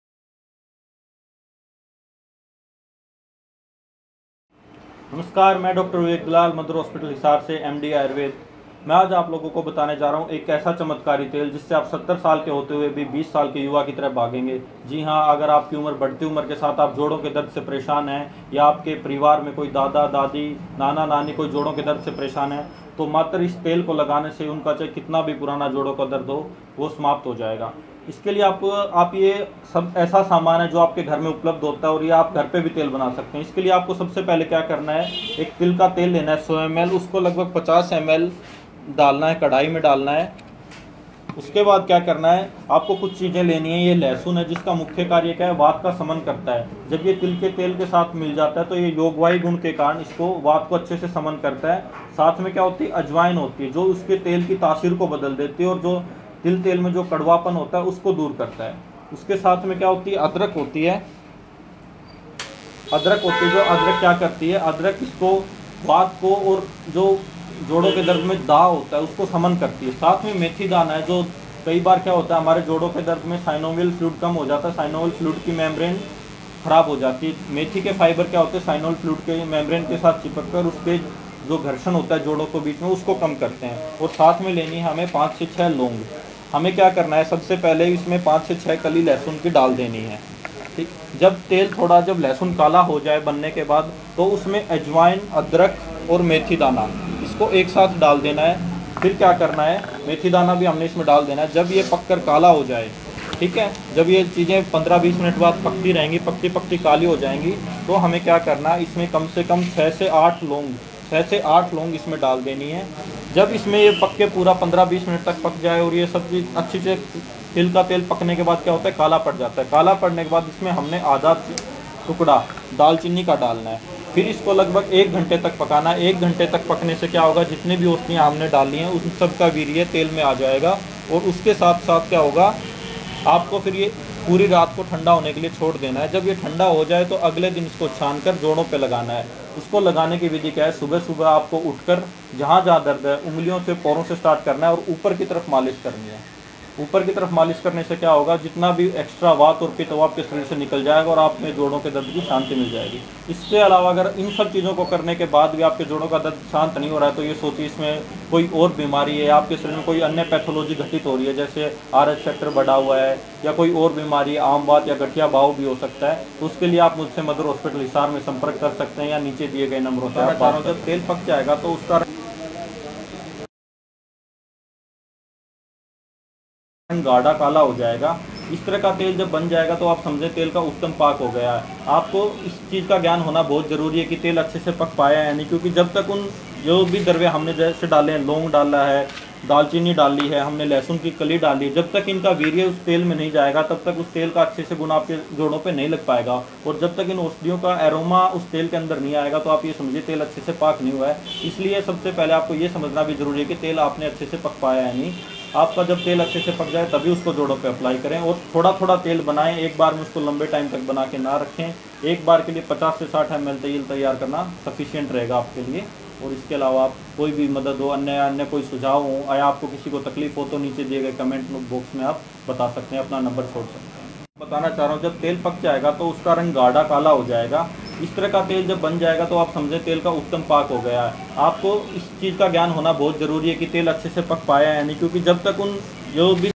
ये चमत्कारी देसी तेल अपनाने से 70 साल के बुजुर्ग भी 20 साल के युवा की तरह भागेंगे, आ जाएगी फुर्ती
ये चमत्कारी देसी तेल अपनाने से 70 साल के बुजुर्ग भी 20 साल के युवा की तरह भागेंगे, आ जाएगी फुर्ती, 18+ युवाओं से लेकर बुजुर्ग ध्यान दें